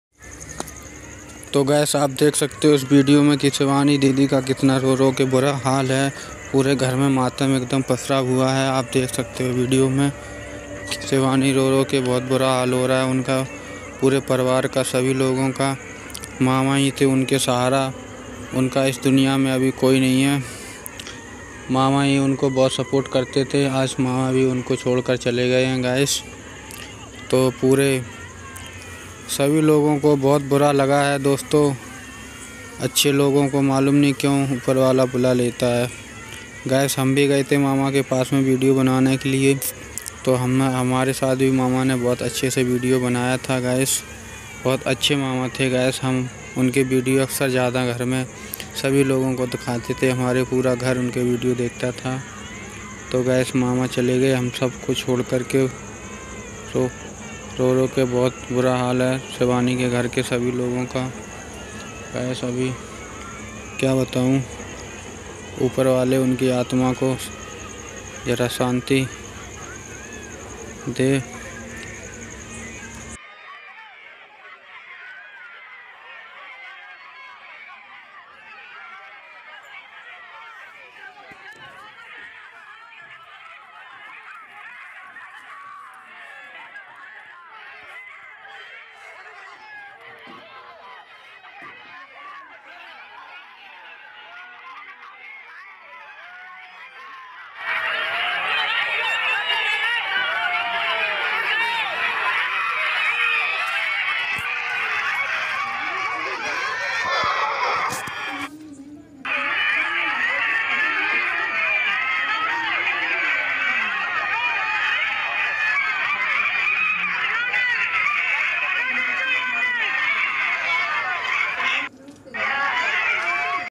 मामा अब नहीं रहे रो रो के बुरा हल शिवानी का
Shivani Kumari ke मामा अब नहीं रहे